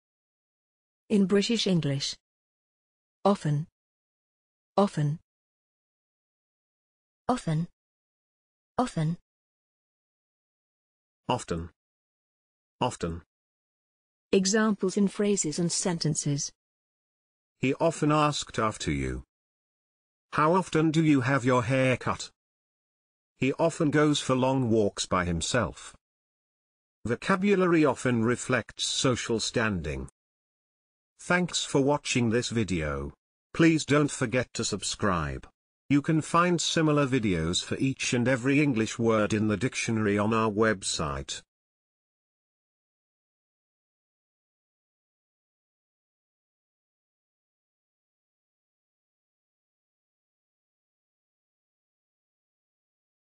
often - pronunciation + Examples in sentences and phrases
This is the pronunciation of often in British English + a few examples of its pronunciation in phrases and sentences.